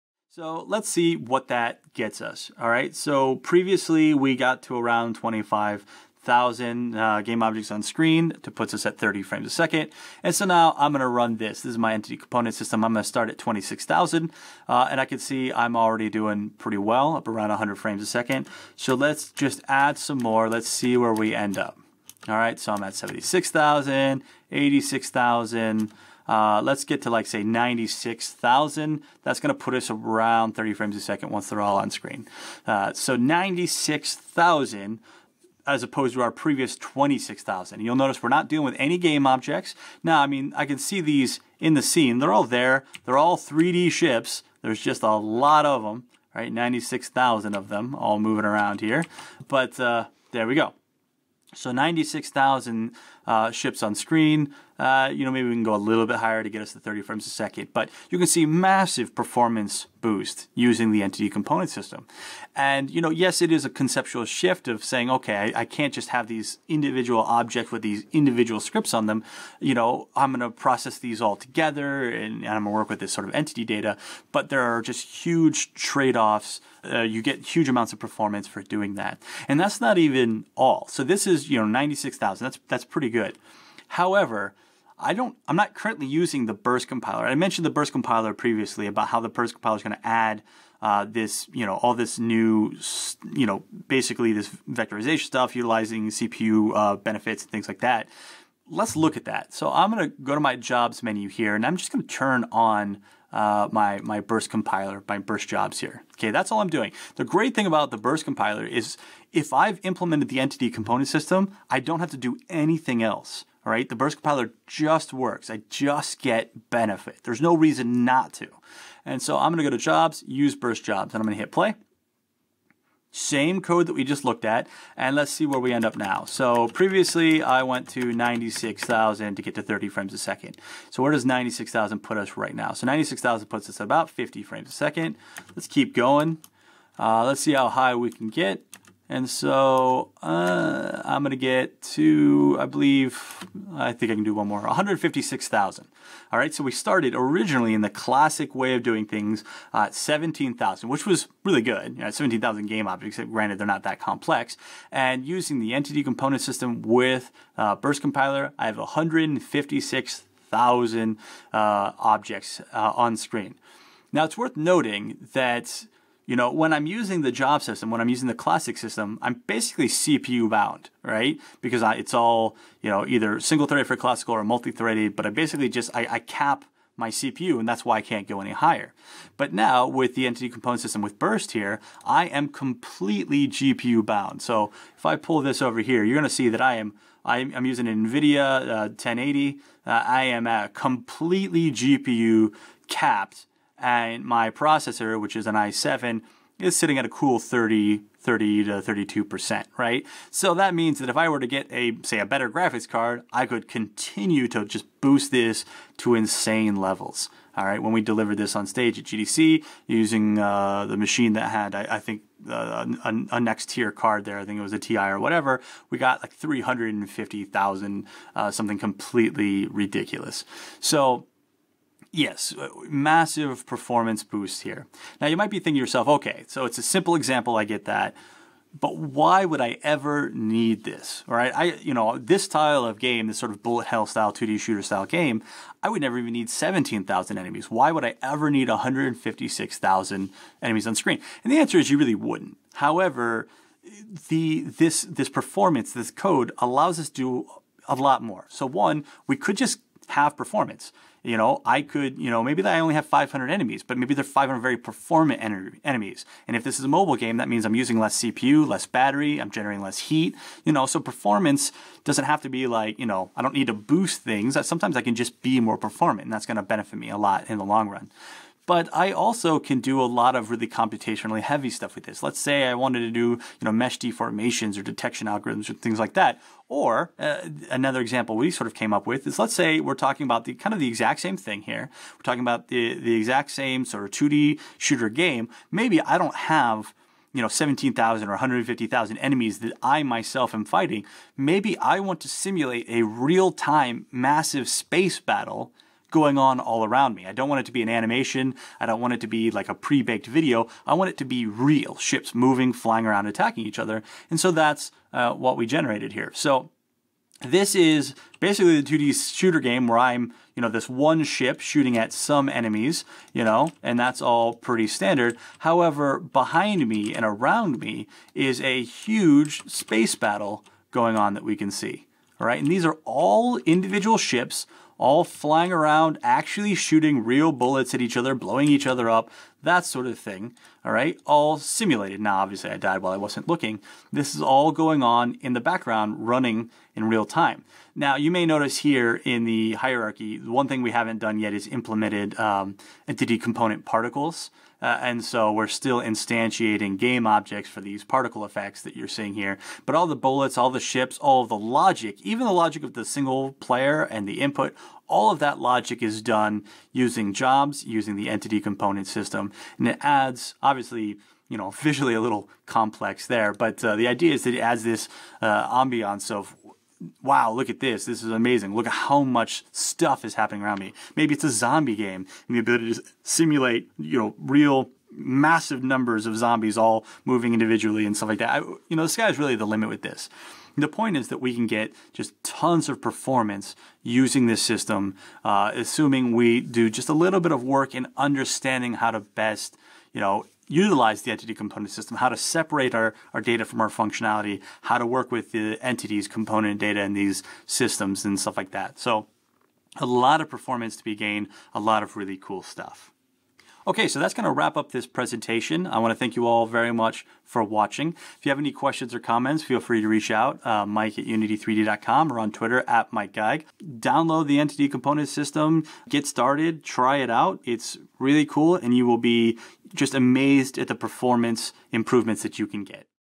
Using The Burst Compiler - Intro To The Entity Component System And C# Job System 5/5
In this presentation, Unity Evangelist Mike Geig explains how to convert a project from Monobehaviour style code to code which uses the C# Job System and then code which uses that and the Entity Component System and Burst Compiler.